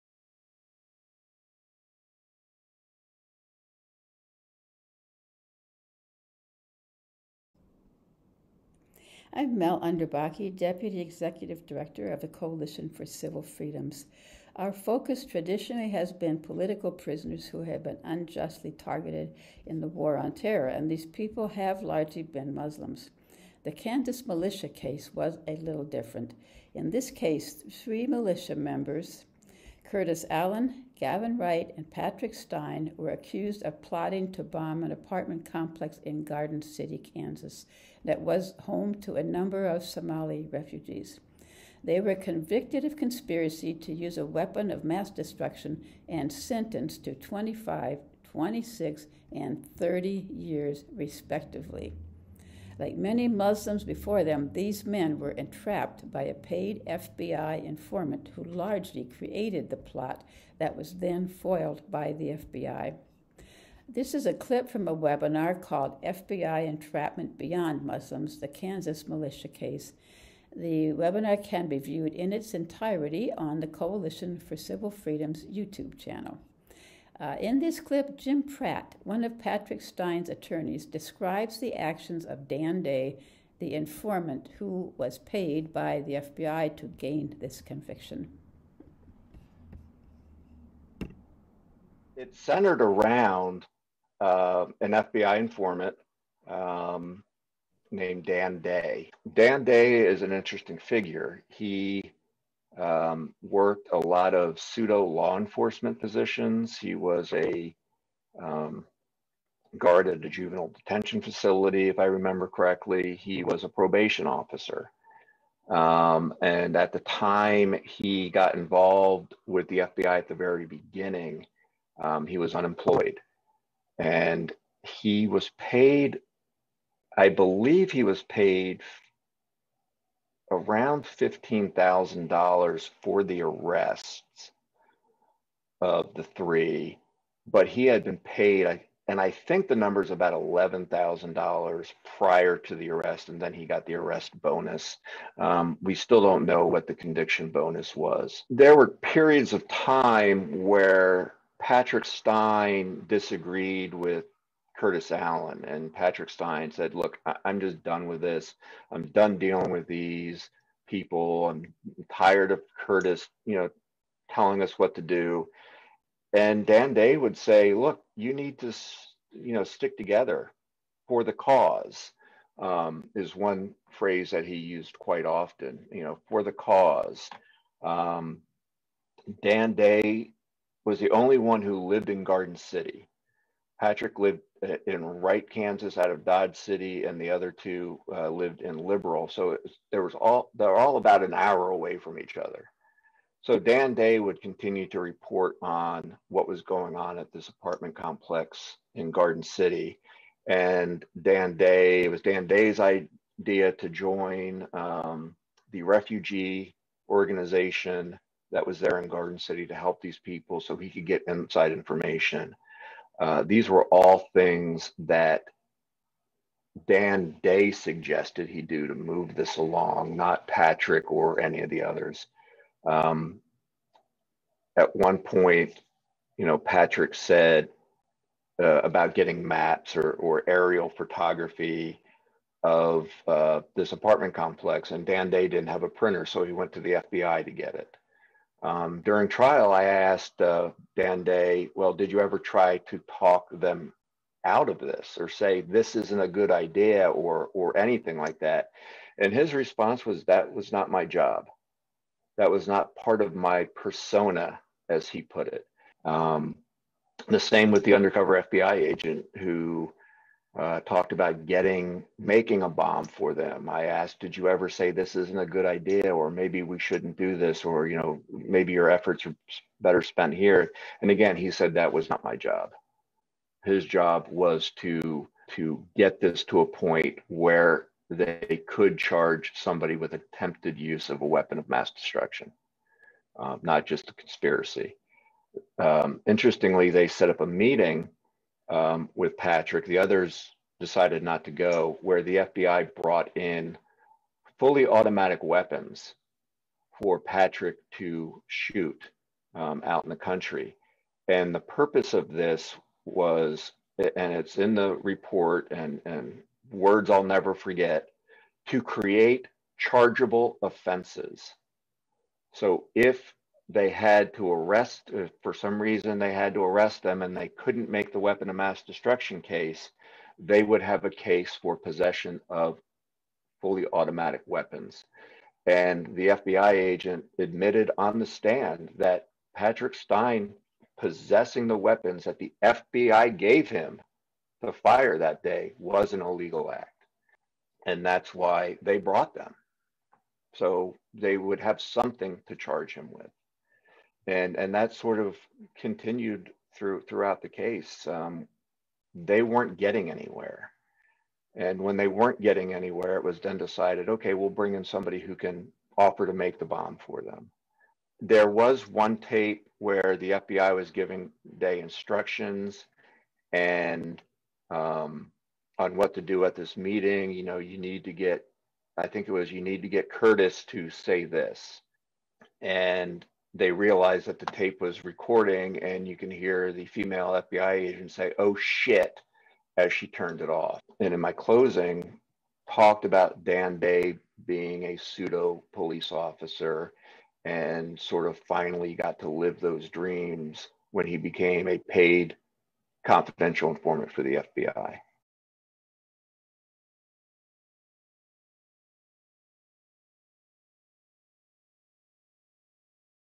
The Informant in Kansas Militia Case
The Informant in Kansas Militia Case. Since 9/11, many Muslims have been entrapped by the FBI. Were these three militia members also entrapped?

Jim Pratt, one of Patrick Stein's attorneys, talks about the informant Dan Day.

(A 7-minute clip from Episode 32 of CCF Webinar series: FBI Entrapment Beyond Muslims: The Kansas Militia Case)